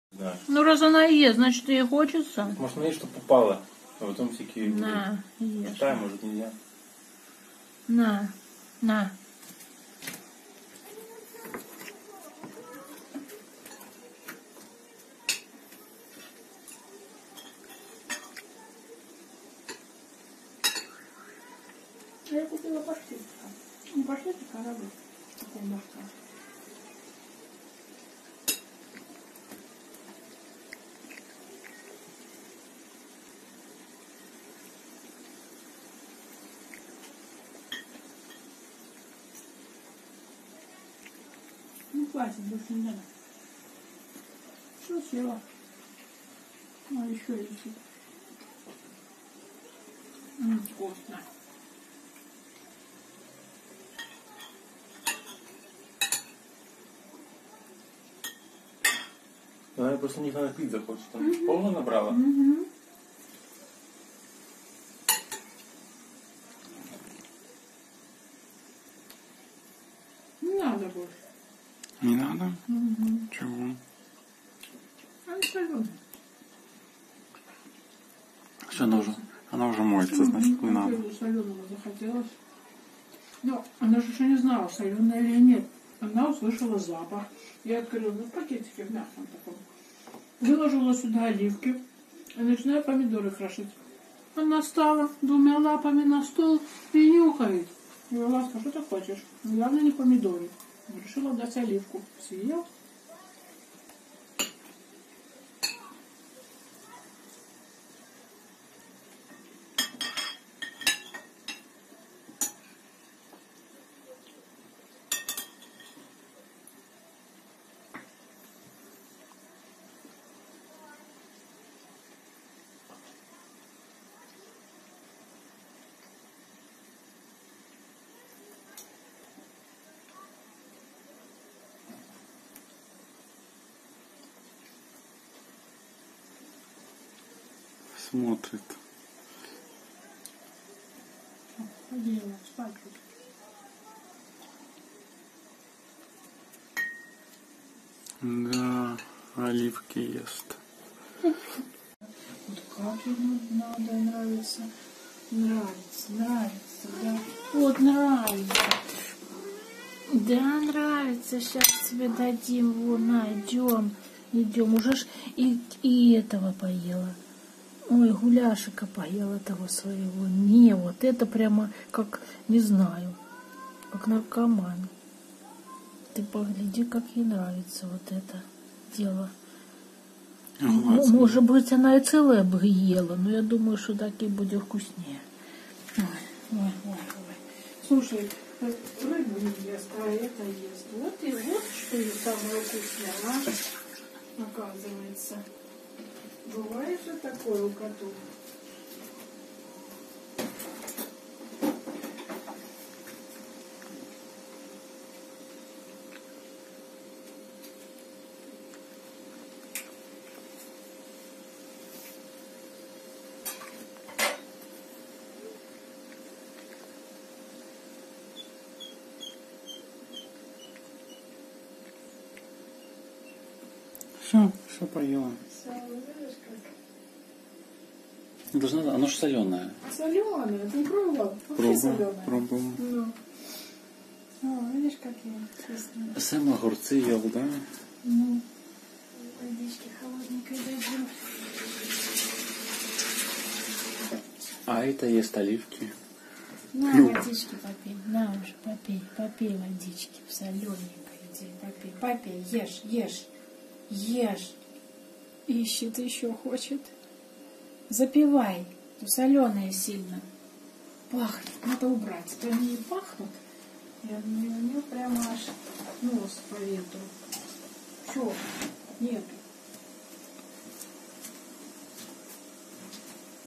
Кошка ест оливки :O
Мы вдруг неожиданно узнали что наша кошка очень любит оливки. Когда мы открыли упаковку с оливками, она стала сильно нюхать и мы её угостили оливками. Ей понравилось, вот и видео про это снял :)